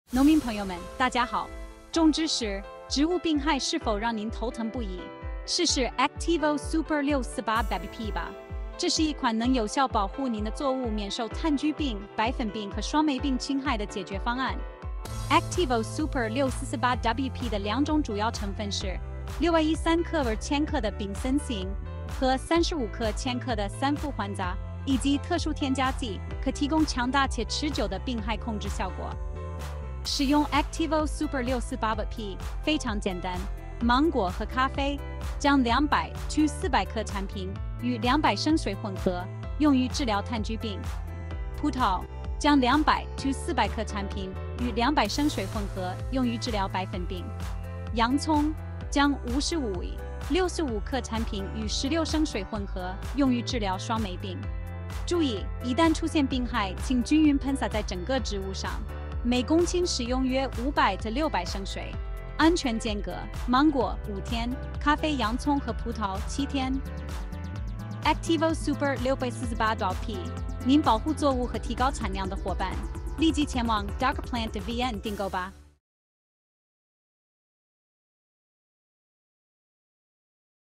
ACTIVO SUPER 648WP -THUỐC TRỪ BỆNH THÁN THƯ, PHẤN TRẮNG, SƯƠNG MAI...hotline:0919817033#bọtrĩ
ACTIVO SUPER 648WP

ĐẶC TRỊ BỆNH THÁN THƯ, PHẤN TRẮNG, SƯƠNG MAI

ACTIVO SUPER 648WP 1

Thành phần của ACTIVO SUPER 648WP

Propineb 613 g/kg

Trifloxystrobin 35 g/kg

Phụ gia 352 g/kg

Công dụng của ACTIVO SUPER 648WP

Đặc Trị Thán Thư Xoài, Phấn Trắng Trên Nho

ACTIVO SUPER 648WP than thu

Đặc tính của ACTIVO SUPER 648WP

– Là thuốc trừ nấm phổ rộng, phòng trừ được nhiều loại nấm hại cây trồng. Là sự kết hợp từ 2 hợp chất trừ nấm nên hiệu quả trừ bệnh cao, hiệu lực kéo dài.

ACTIVO SUPER 648W phan trang

Hướng dẫn sử dụng của ACTIVO SUPER 648W
Xoài, cà phê: Thán thư

Nho : Phấn trắng

200 – 400 g/200 lít nước

Hành: Sương mai: 55 – 65 g/bình 16 lít nước

Lưu ý:

– Phun ướt đều cây trồng khi bệnh chớm xuất hiện.

– Lượng nước phun từ 500 – 600 lít/ha.

Thời gian cách ly:Xoài – 5 ngày; cà phê, hành và nho – 7 ngày.